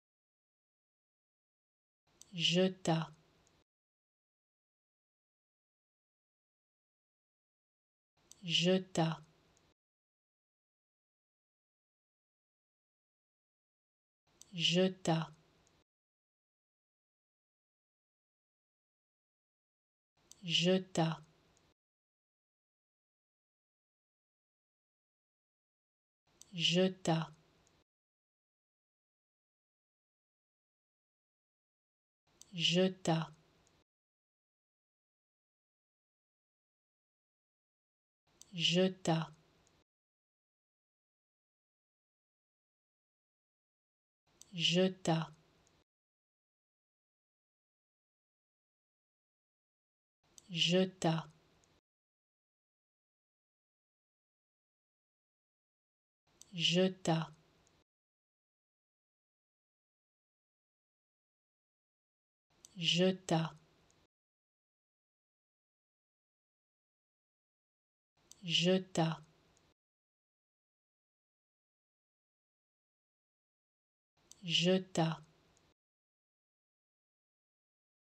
How to Pronounce ''Jeta'' Correctly in French
Learn how to say and properly pronounce ''Jeta'' in French with this free pronunciation tutorial. Apprenez à prononcer des mots en français grâce à nos tutoriels.